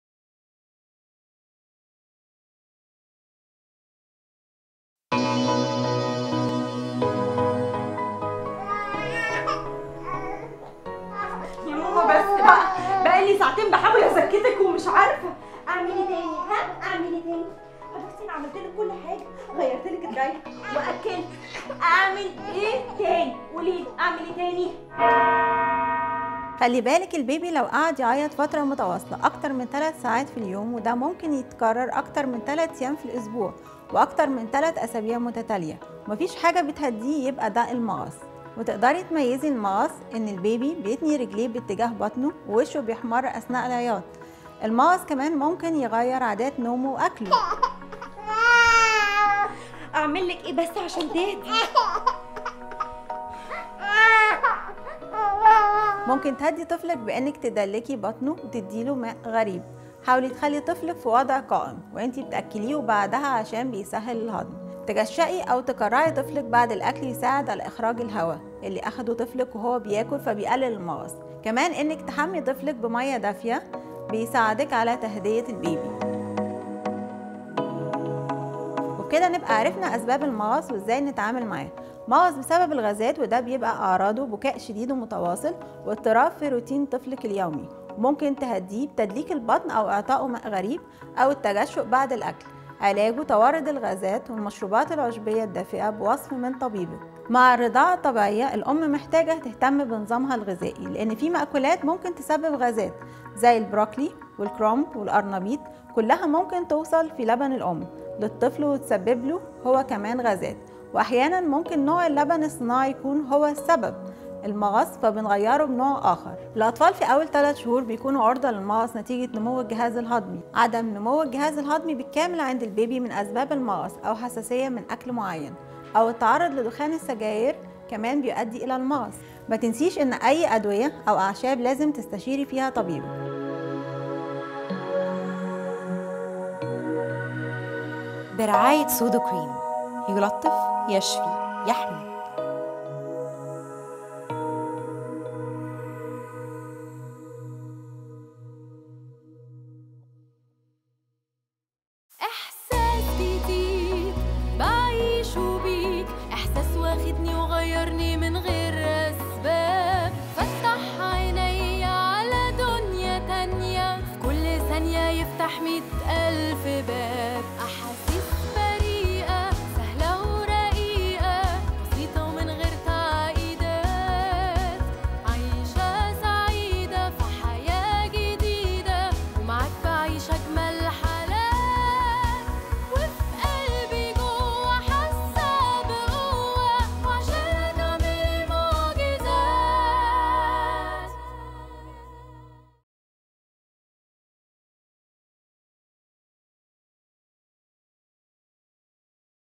Sudocrem | Epsiode (2) - Arabic
• Description: A 3-Episodes series with our client Sudocrem featuring the cream and its importance in looking after your baby’s delicate skin and helping to soothe sore skin and treat nappy rash.
• Project: Episode (2) - Arabic
• Client: Sudocrem
• Agency: Hive Studio
• Style: Video Production
• Country: Egypt